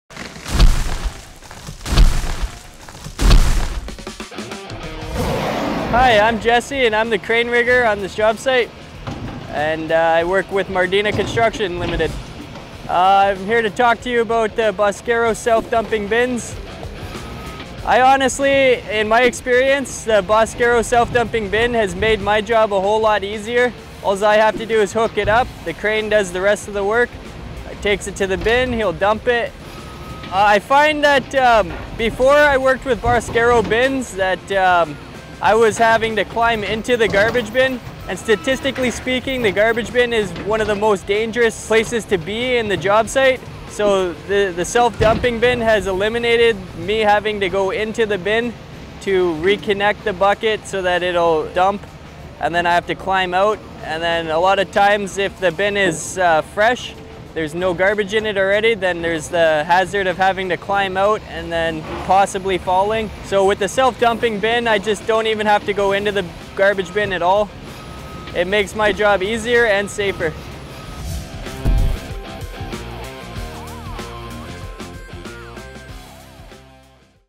Jesse Talks Self Dumping Bins
Jesse talks real world safety and how Self Dumping Bins from Boscaro make him safer and more productive.